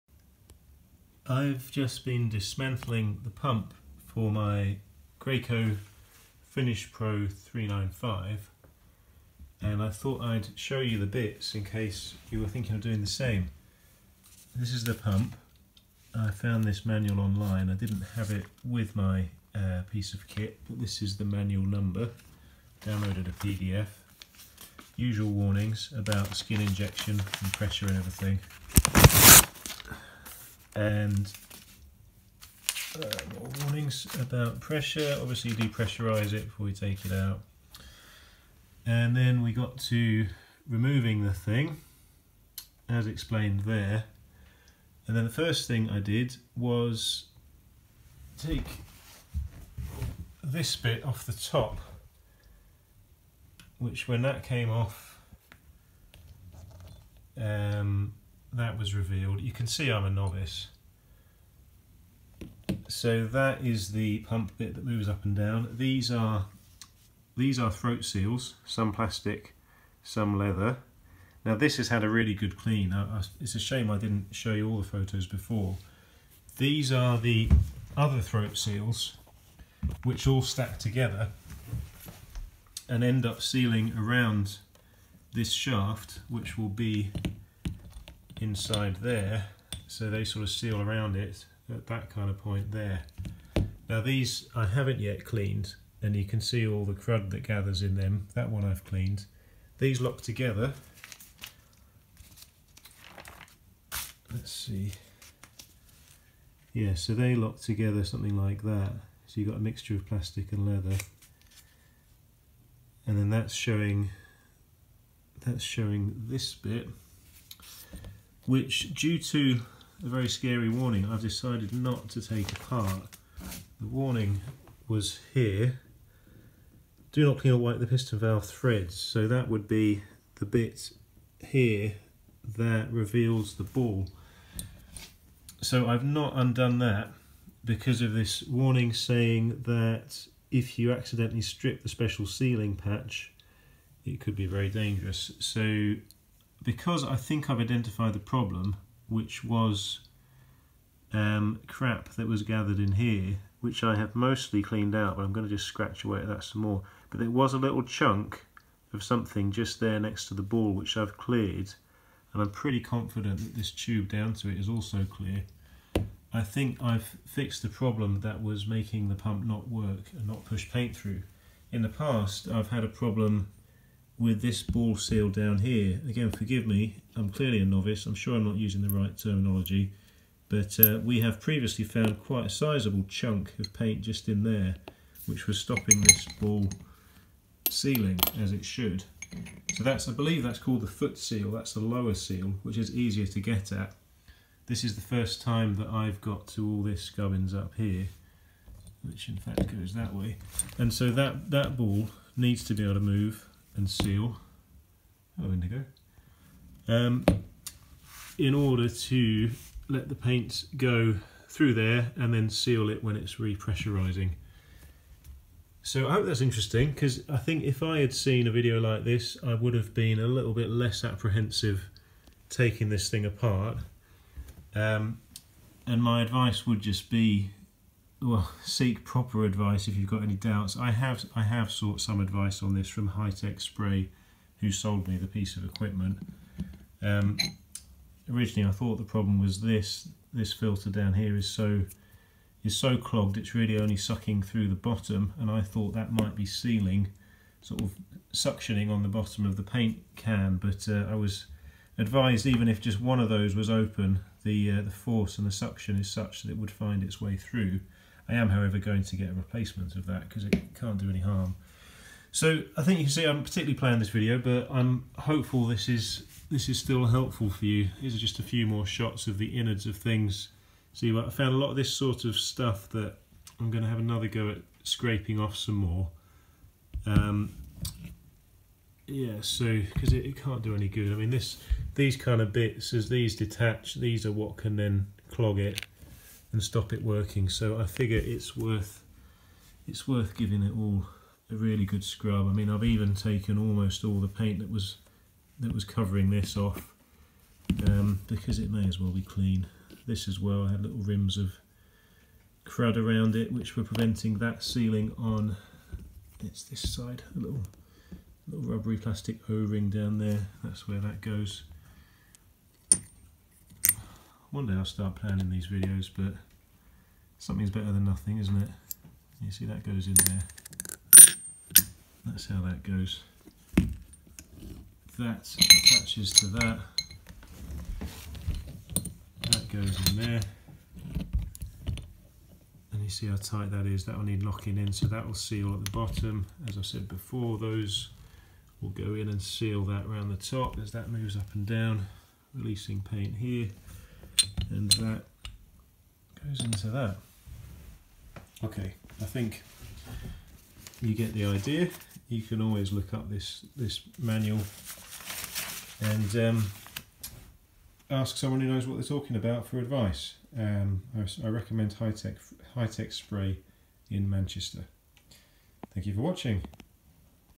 Dismantling pump for Graco FinishPro II 395 Air Assisted Airless spray painting machine
Learning as I go along, I decided to dismantle the pump (with the help of PDF manual no. 334599F and Graham from Hi-Tec Spray in Manchester on the phone).  The pump was not drawing paint even though it was audibly running and it was still running even after the trigger of the G40 gun was released.  In the past a similar problem was due to the ball valve at the bottom (the foot valve I think Graham called it, or ‘intake valve’ in the  manual) having a sizeable piece of crud blocking it.  This time I had to hunt a bit further and the problem seemed to be a similar issue at the upper ball valve or piston valve.   I am giving the whole thing a thorough clean and what you see here is much cleaner than it started.   We have been using exclusively water based (acrylic) paints and are just in the process of transitioning to ‘decorators’ paints’ - Leyland and Johnstone’s acrylic eggshell - to more suitable proper spraying paint.  We chose Movac’s Sayerlack water based.   We are spraying parts for fitted furniture, mostly white MDF.  Cameo from Indigo the cat.